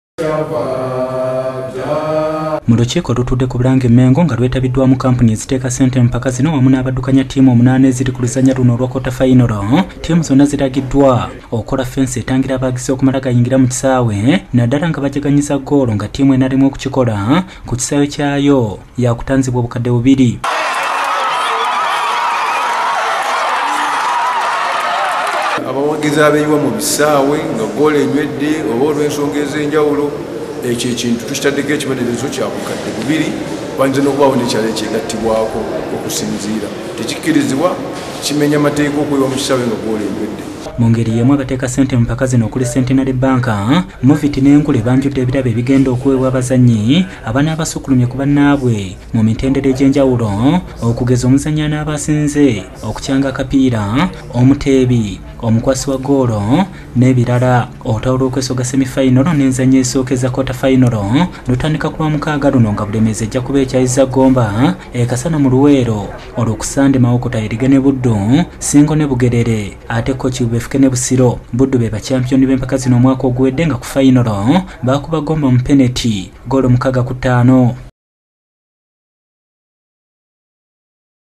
SPORTS:Ttiimu eziri ku Quarter ziweereddwa amateeka ampya
"Abawagizi okuyingira mu kisaawe,atanzibwa bukadde 2"

Reporter: